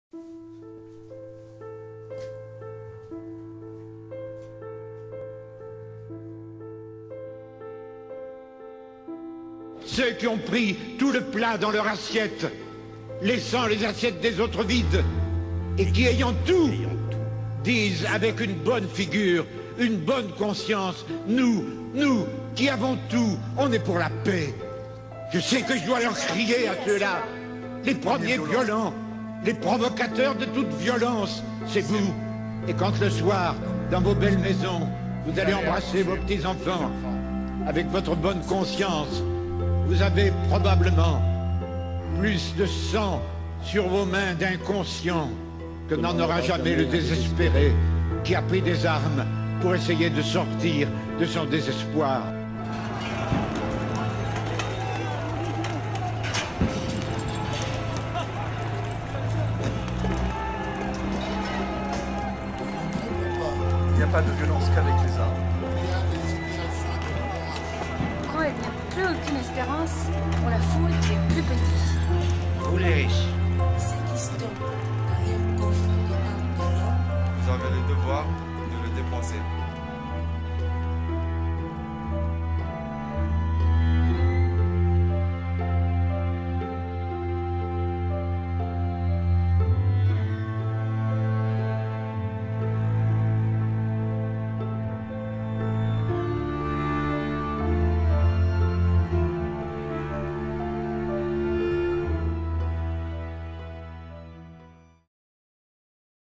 The voice of the voiceless (english)
We need to declare war against poverty, and not those who fight for justice and dignity around the world. As Abbé Pierre’s universal legatee, Emmaus International has not forgotten the words of its founder, which still resonate due to their relevance in France, in Europe and throughout the world.

This film exists thanks to the collaboration of the following french Emmaus communities  : Saint-Etienne, Bourgoin-Jallieu, Lyon, Sevras, Tarare, Annemasse, Beaugies-sous-Bois, Chambery, Vienne, Bourg-en-Bresse et Saint-Paul-Les-Romans.

A film directed by @LaSecte agency.